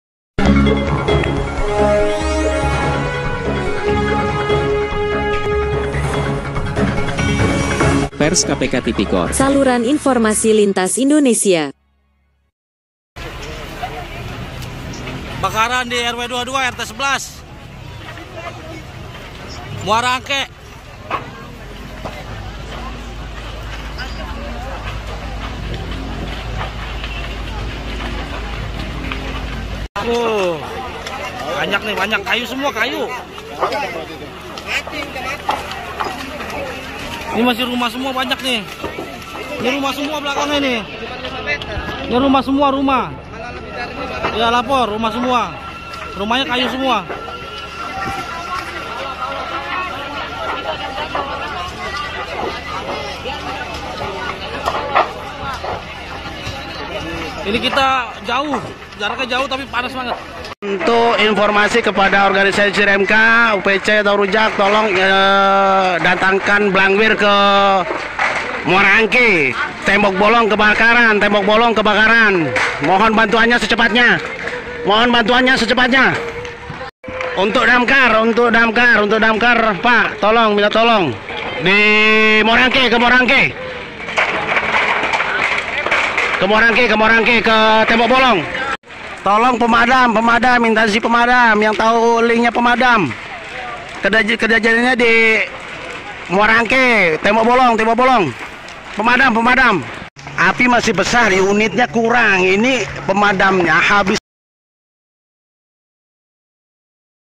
*DI TINGGAL MUDIK LEBARAN KEBAKARAN DI PEMUKIMAN PADAT PENDUDUK MUARA ANGKE JAKARTA UTARA*
Jakarta,- perskpktipikor.com.

Peristiwa kebakaran terjadi saat di tinggal mudik Lebaran oleh sebagian warga masyarakat di pemukiman padat penduduk pada Hari/Tanggal, Sabtu, 22 April 2023 Pukul: 01.50 WIB WIB. Di Jl. Muara Angke Blok Empang RT.011/RW.022, Kel. Pluit, Kec. Penjaringan,Jakarta Utara.

Data sementara yang berhasil di kumpulkan
Untuk wilayah yang terdampak adalah di Rw.022 Rt.011 Jl.Muara Angke Jakarta Utara,sebagian objek yang terdampak kebakaran adalah Rumah Tinggal Semi Permanen kurang lebih 100 Rumah dan 150 kk, untuk korban jiwa dan materi masih dalam pendataan,

Untuk kronologis kejadian masih dalam penyelidikan dan pengembangan oleh pihak-pihak terkait dan berwajib di lokasi kebakaran.

Saat ini jumlah penyintas di lokasi pengungsian terdapat kurang lebih 100 jiwa
Sebagian Warga yang terdampak sedang mudik Ke Kampung Halaman Sehingga tumah tinggal Kosong.

Saat kejadian terjadi SKPD terkait yang hadir datang di tempat kejadian adalah:
- 22 Unit DAMKAR
- 1 Unit TRC BPBD
- TNI POLRI
- PLN,PMI,AGD
Saat ini di lokasi pengungsian ada kebutuhan Mendesak :
- Tenda Pengungsian,Genset
- Air Mineral
- Matras
- Selimut
- Makanan Siap Saji

Bagi masyarakat sekitar Kel.Pluit kec.Penjaringan yang tidak terdampak kebakaran ingin membantu dan salurkan donasi kebutuhan untuk ringankan beban korban kebakaran dapat menghubungi Bpk. Bani Sadar (Ketua Rw 022) Hp : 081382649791,ungkapnya.

Indra perskpktipikor